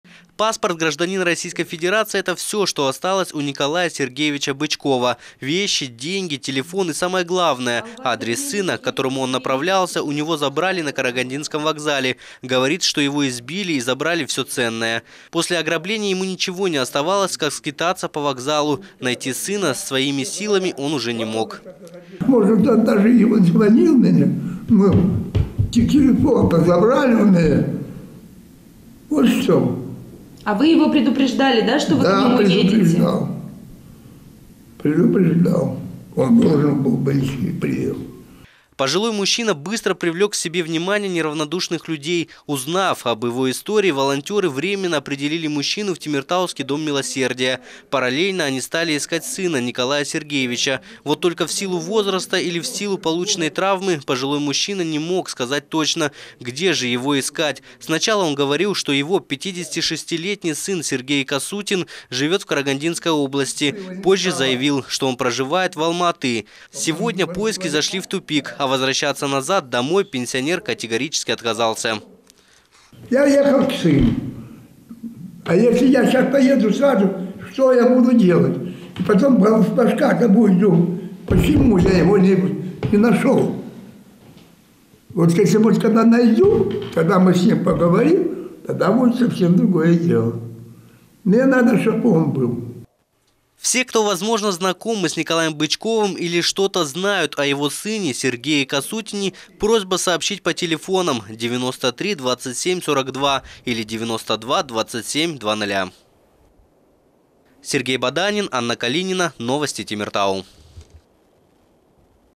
Потерялся мужчина
Волонтёры пытаются найти родственников 81-летнего россиянина, который, по всей вероятности, стал жертвой преступников на карагандинском вокзале. Пожилого мужчину избили и ограбили когда он направлялся в гости к своему сыну Сергею Касутину. Вместе с ценными вещами пропал и адрес сына, который сейчас пенсионер никак не может вспомнить.